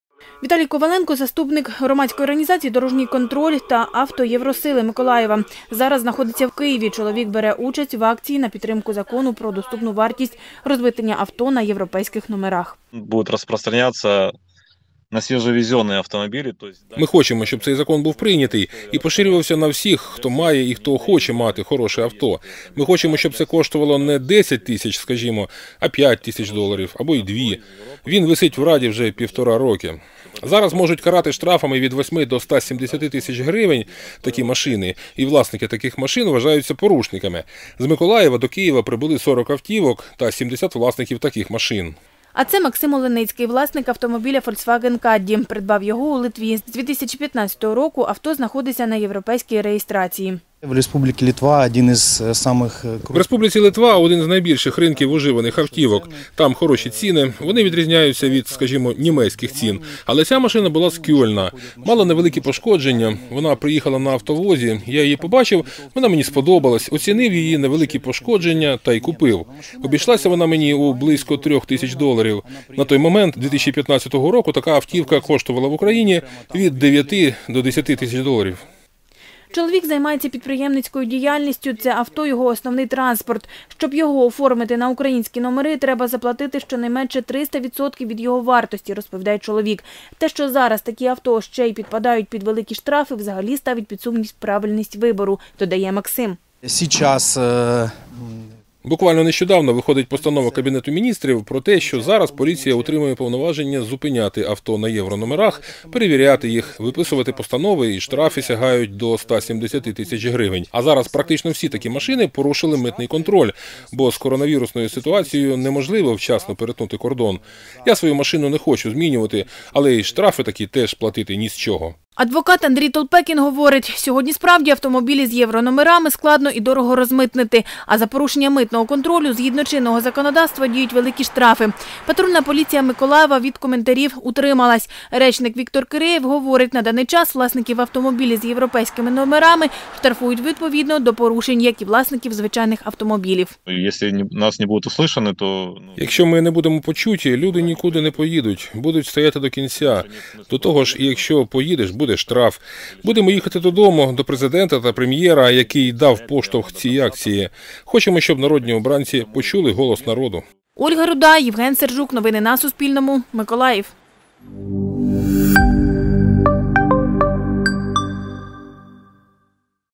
Миколаївці долучились до акції протесту власників автівок на єврономерах
Під стінами Верховної Ради України відбулась акція власників автівок, які зареєстровані на європейських номерах. Люди вимагають прийняти закони, які зменшать вартість розмитнення таких авто. Із Миколаєва приїхало 70 людей на сорока автомобілях.

Віталій Коваленко ‒ заступник громадської організації “Дорожній контроль” та “Автроєвросила Миколаєва” був у Києві.  Чоловік брав участь в акції на підтримку закону про доступну вартість розмитнення авто на європейських номерах.

Станом на 3 листопада, справді автомобілі з єврономерами складно і дорого розмитнити, а за порушення митного контролю, згідно чинного законодавства, діють великі штрафи. Патрульна поліція Миколаєва від коментарів утрималась. Речник Віктор Кірєєв говорить, що власників автомобілів із європейськими номерами штрафують відповідно до порушень, як і власників звичайних автомобілів.

Авторка ‒ Ольга Руда, оператор ‒ Євген Сержук.